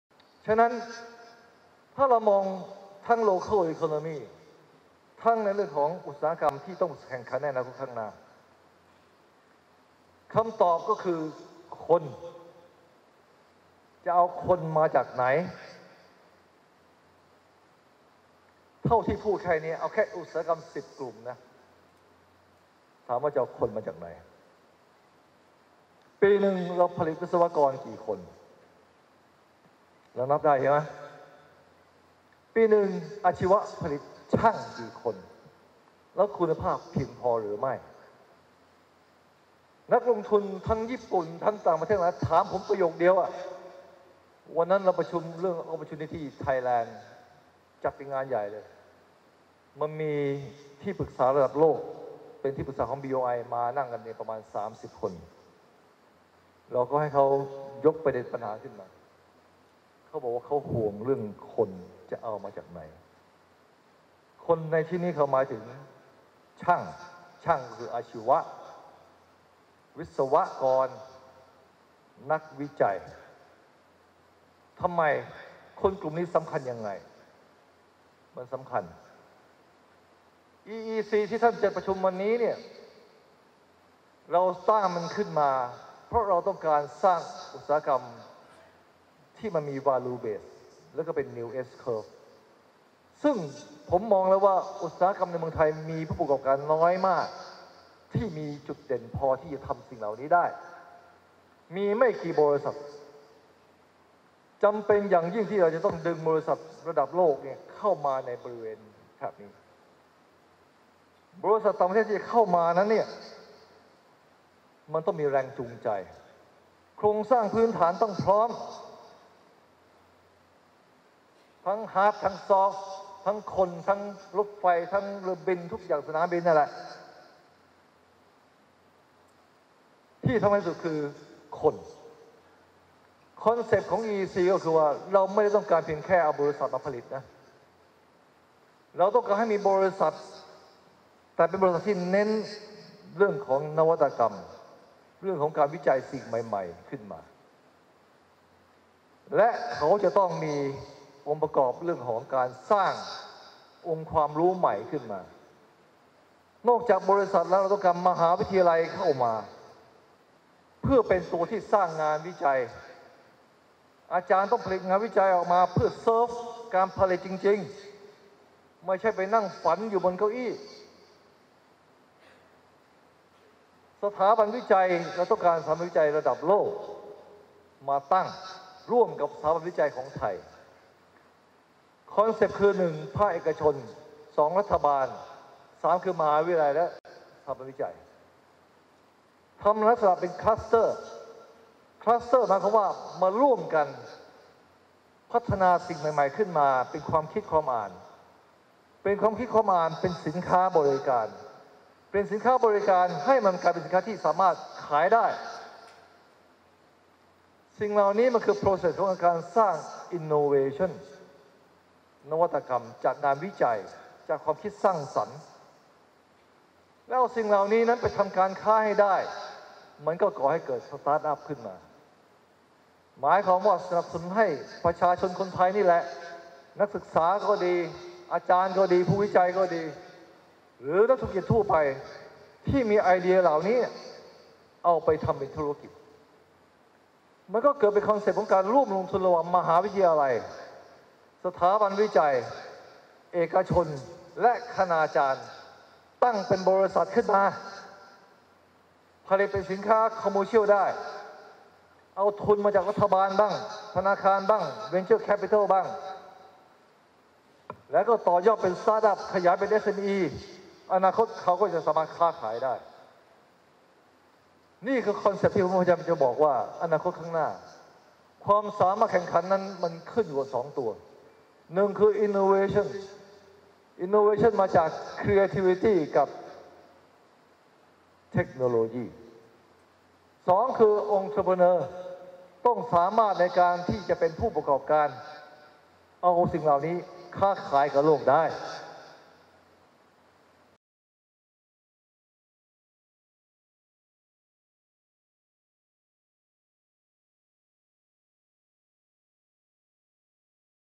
อุตสาหกรรมอนาคต คนคือหัวใจสำคัญ
ดร.สมคิด จาตุศรีพิทักษ์ รองนายกรัฐมนตรี กล่าวในงานปาฐกถาเรื่อง “ปฏิรูปการศึกษา สร้างคน รับระเบียงเศรษฐกิจ” โดยกล่าวว่า การพัฒนาประเทศทั้งในฝั่งของ Local economy และอุตสาหกรรมที่ต้องแข่งในโลกข้างหน้า คำตอบคือคน ที่ผ่านมาปริมาณการผลิตวิศวกรและช่างไม่เพียงพอที่ปริมาณและคุณภาพ ต่างชาติที่จะมาลงทุนให้ความสำคัญมากว่าไทยจะผลิตคนกลุ่มนี้รวมทั้งนักวิจัยอย่างเพียงพอหรือไม่ ในอีอีซีเราจำเป็นต้องดึงบริษัทระดับโลกเข้ามา ซึ่งคนเหล่านี้จะเข้ามาได้ ปัจจัยในการตั้งฐานการผลิตต้องพร้อม  องค์รวมของอีอีซีนอกเหนือจากการมีบริษัทผลิตแล้วต้องมีหน่วยงานวิจัย มีสถาบันการศึกษาป้อนบุคลากรและงานวิจัย มีการครัสเตอร์หน่วยงานต่างๆ เพื่อสร้างนวัตกรรม แล้วสามารถนำไปขายได้ สร้างเป็นธุรกิจได้ ความสามารถการแข่งขันอนาคตได้ขึ้นอยู่กับนวัตกรรม ขีดความสามารถของผู้ประกอบการนำสินค้านวัตกรรมไปขายกับโลกได้